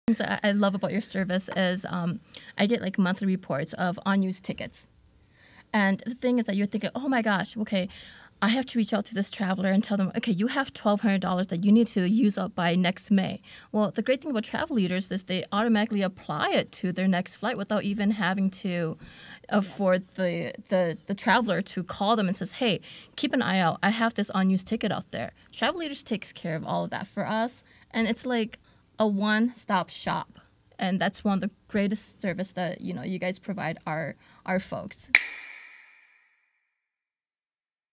ONE-STOP-SHOP | TRAVEL MANAGEMENT COMPANY
Who is your Travel Management Company? Our team sat down and discussed the services we offer when it comes to serving businesses and maximizing their travel spend.